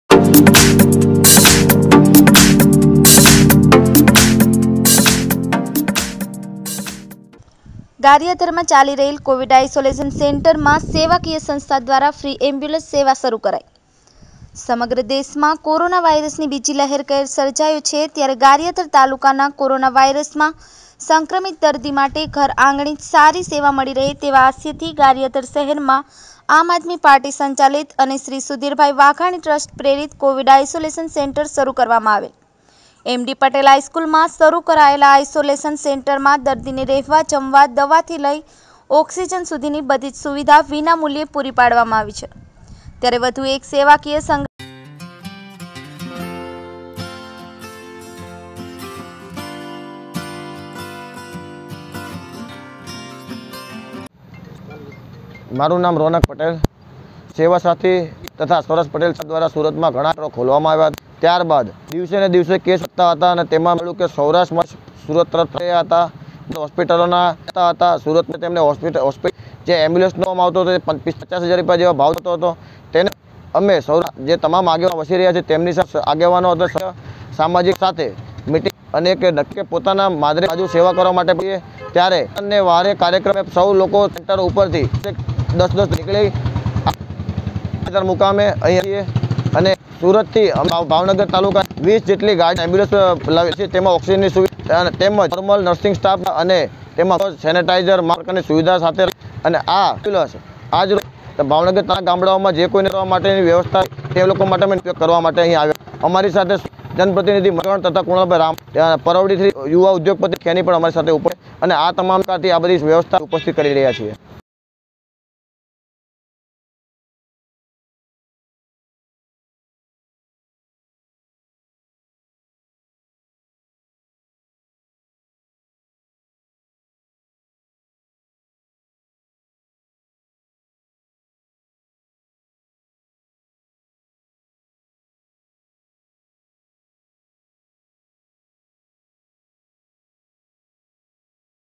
GARIYADHARમાં ચાલી રહેલ કોવિડ આઇસોલેશન સેન્ટરમાં સેવાકીય સંસ્થા દ્વારા ફ્રી એમ્બ્યુલન્સ સેવા શરૂ...
ગારીયાધાર માં ચાલી રહેલ કોવિડ આઇસોલેશન સેન્ટરમાં સેવાકીય સંસ્થા દ્વારા ફ્રી એમ્બ્યુલન્સ સેવા શરૂ કરાઈ

સમગ્ર દેશમાં કોરોના વાઈરસની બીજી લહેરે કહેર સર્જયો છે ત્યારે ગારીયાધાર તાલુકા ના કોરોના વાઇરસ માં સંક્રમિત દર્દી માટે ઘર આંગણે જ સારી સેવા મળી રહે તેવા આશય થી ગારીયાધાર શહેરમાં આમ આદમી પાર્ટી સંચાલિત અને શ્રી સુધીરભાઈ વાઘાણી ટ્રસ્ટ પ્રેરિત કોવિડ આઇસોલેશન સેન્ટર શરૂ કરવામાં આવેલ એમ ડી પટેલ હાઇસ્કુલ માં શરૂ કરાયેલ આ આઈસોલેશન સેન્ટર માં દર્દીને રહેવા, જમવા, દવા, થી લઇ ઑક્સિજન સુધીની બધીજ સુવિધા વિના મૂલ્યે પૂરી પાડવામાં આવી રહી છે

ત્યારે વધુ એક સેવાકીય સંગઠન લોકોની મદદે આગળ આવ્યુ છે સુરત થી સુદામા ગ્રુપ અને સેવા સંસ્થા દ્વારા ગારીયાધાર એમ ડી પટેલ સ્કૂલ ખાતે ચાલી રહેલા covid isolation સેન્ટરમાં એમ્બ્યુલન્સ રૂપે નિશુલ્ક સેવા માટે આપવામાં આવનાર છે સુરતથી આવેલા આ યુવાનોએ કુલ ત્રણ એમ્બ્યુલન્સ જેમાંથી એક એમ્બ્યુલન્સ ભાવનગર ખાતે એક એમ્બ્યુલન્સ ગારીયાધાર ખાતે અને એક એમ્બ્યુલન્સ જૂનાગઢ ખાતે સેવા અર્થે આપવામાં આવેલ છે બેઝિક લાઇફ સપોર્ટ સિસ્ટમ માટે જરૂરી ઓક્સિજન અને ફ્લોમીટર લગાવવામાં આવેલ છે તથા કોરોના માટે જરૂરી દવાઓ, નર્સિંગ સ્ટાફ, સેનેટાઇજર સહિતની સુવધાઓ સાથે આ એમ્બ્યુલન્સમાં રાખવામાં આવેલ છે જેના કારણે ગારિયાધારના અને આજુ બાજુના ગ્રામ્ય વિસ્તારના લોકો ને અન્ય જગ્યાએ રીફર કરવામાં વિના મૂલ્યે ચાલતી આ એમ્બ્યુલન્સ મદ રિપોર્ટ અશ્વિન કણસાગરા ગારીયાધાર